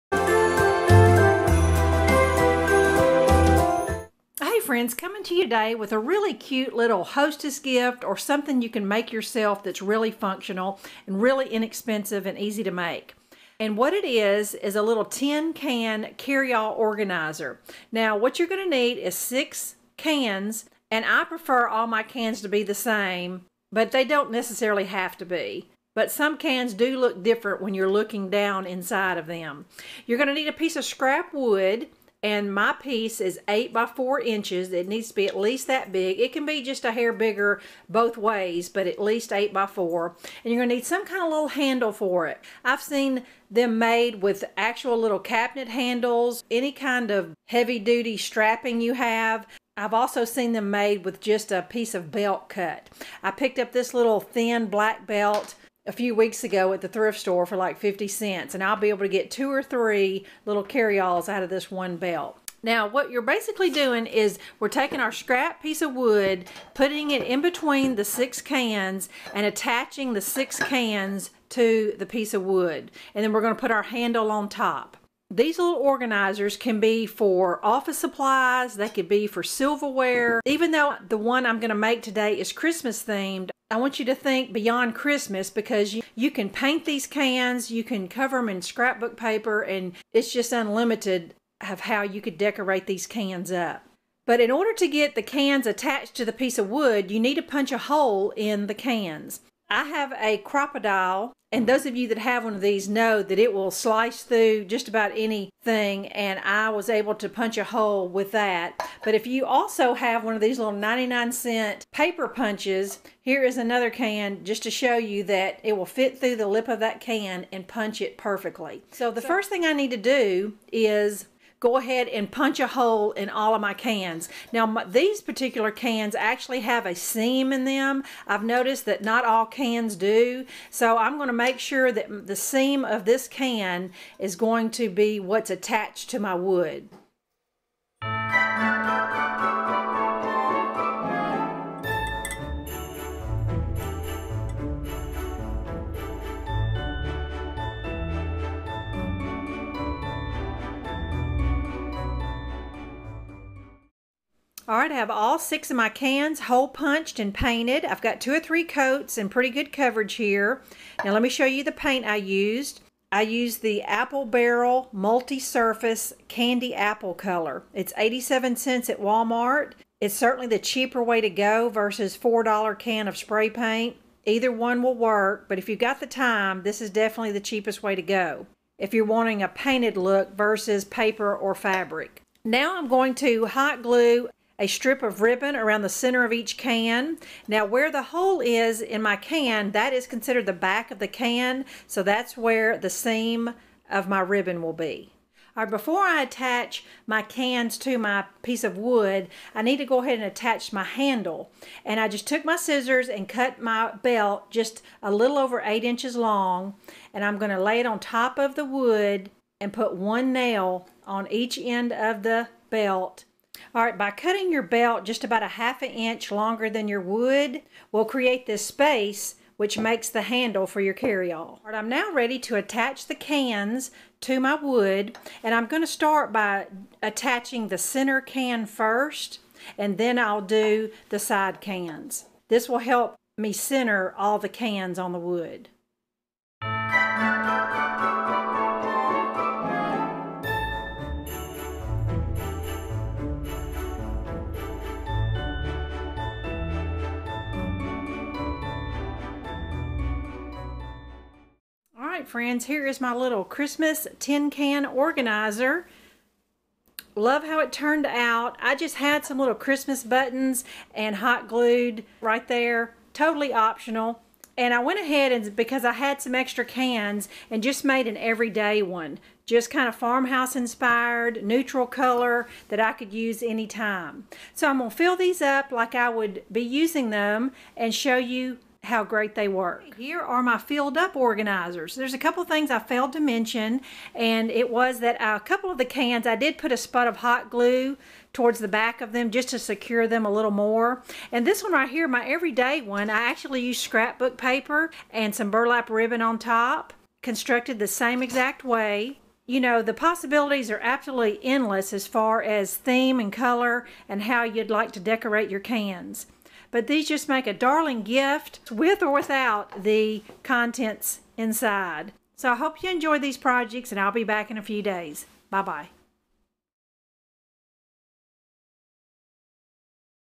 Tin Can Organizer Carry All
Create this tin can organizer for a gift or yourself.  Its a quick inexpensive project most everyone could use.  Personalize the cans to fit home decor or sports team .  The possibilities are endless for a one of a kind gift.